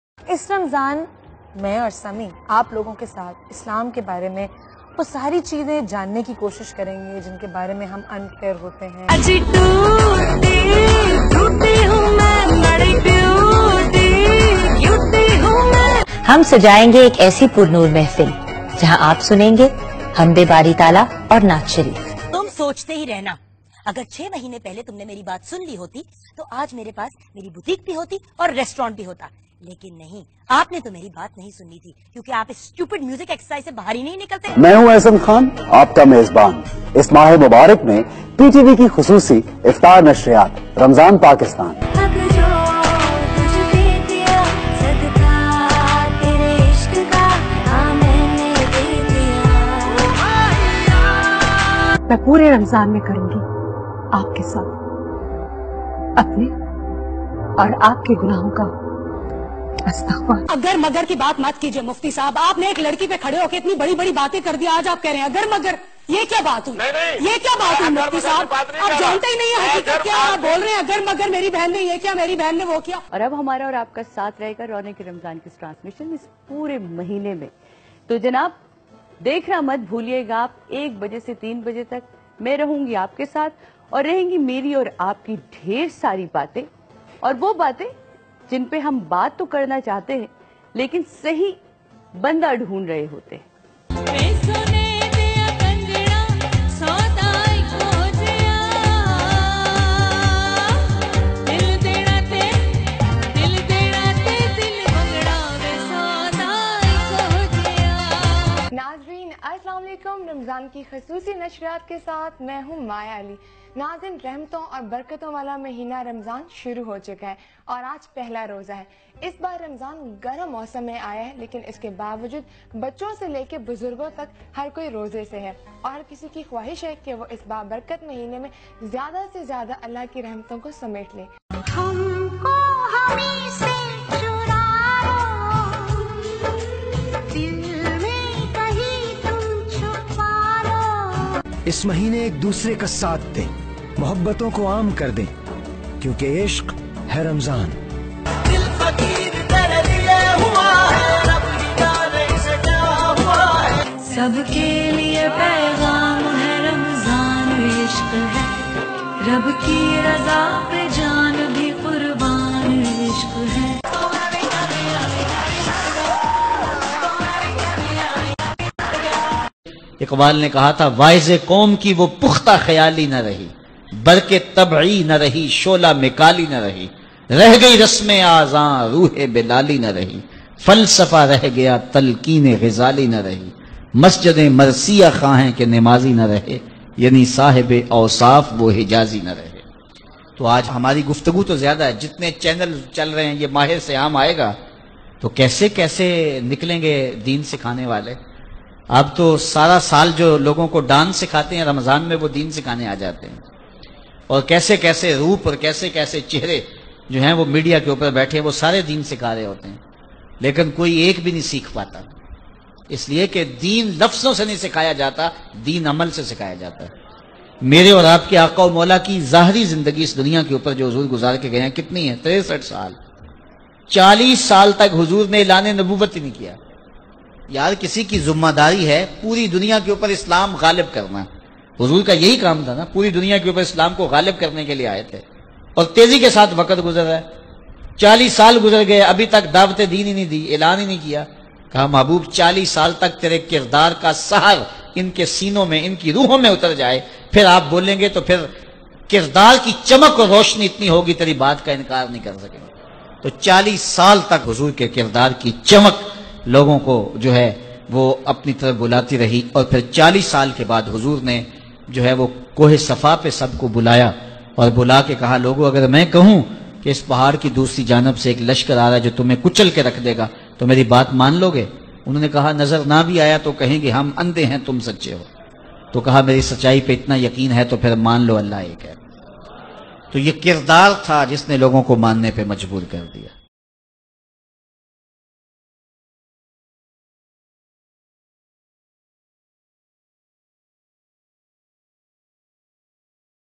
Reality of Pakistani actor/actress before and after Ramadan
Watch the reality of all Pakistani actors before and after ramadan. Watch top actor sadia imam, ahsan khan, maya ali, sahir lodhi, veena malik & ayesha omer, changing their behaviors from end of the extreme to another during month of ramadan.